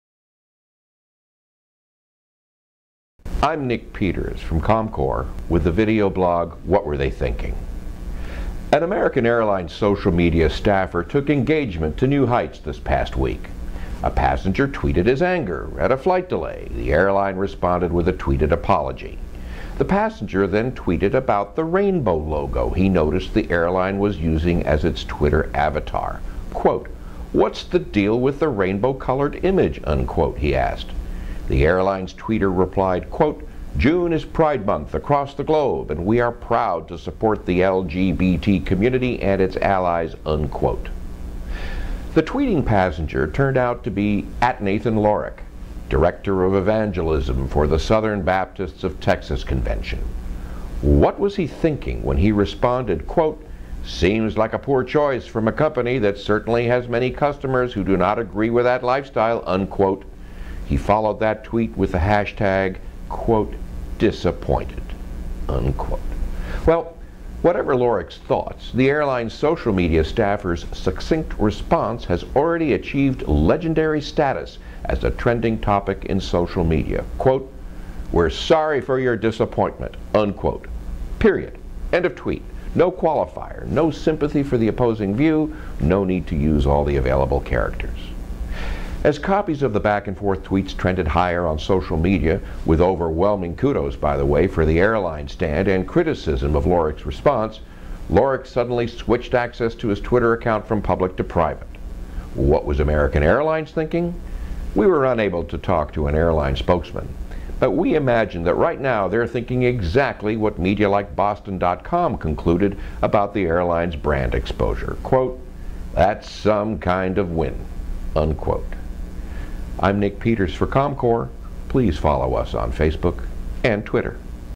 What Were They Thinking: American Airlines Strikes Gold at the End of the Rainbow
An angry tweet about a flight delay turns into an issue on homosexuality when passenger, Nathan Lorick, questions American Airlines’ use of a rainbow colored image as its Twitter avatar.